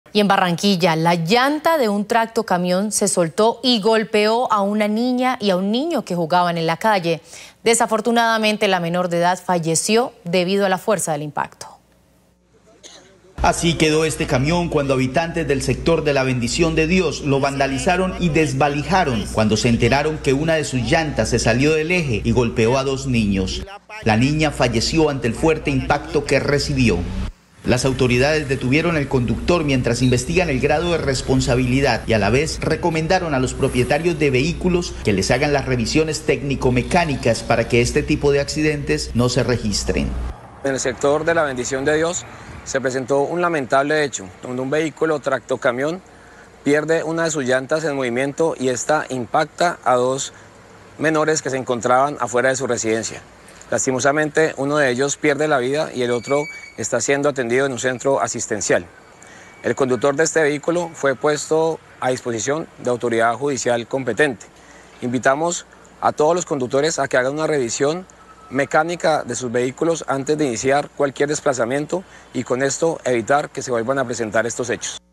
Llanta de un tractocamión que se soltó mató a una niña
La llanta de un tractocamión que se soltó mató a una niña y dejó a otro menor herido en el barrio la Bendición de Dios, Barranquilla.